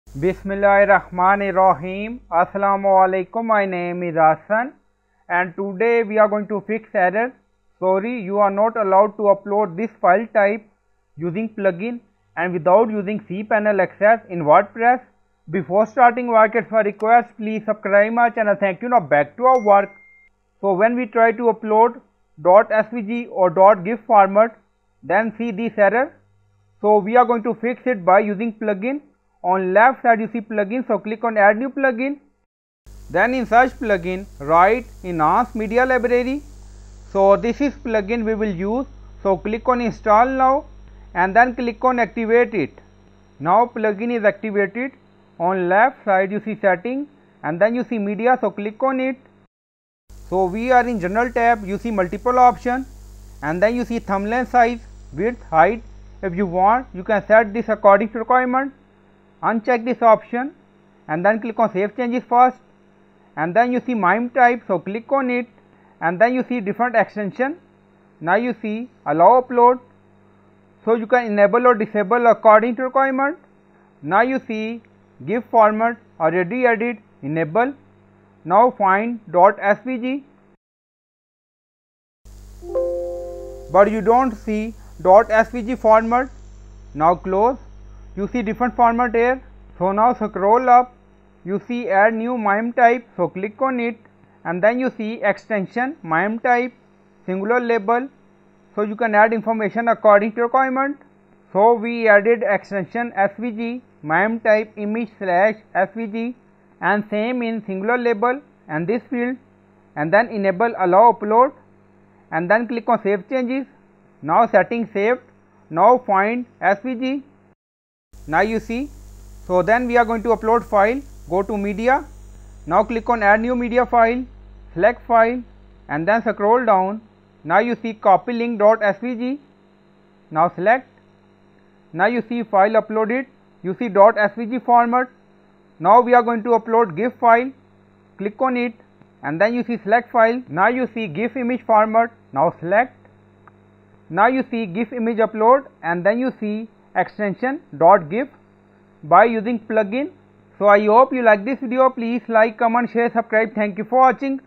How to Fix Sorry You are not Allowed to Upload this File Type using Plugin Without cPanel WordPress
In this wordpress tutorial for beginners you will learn how to fix error sorry, you are not allowed to upload this file type using plugin " Enhanced Media Library " in wordpress website. So you can fix this issue without adding code in wp-config.php file in cpanel. So after that you can upload / add any file type e.g. .svg format (SVG), .gif image (GIF), .ttf file type and if you want to you can create new mime types with extension in plugin.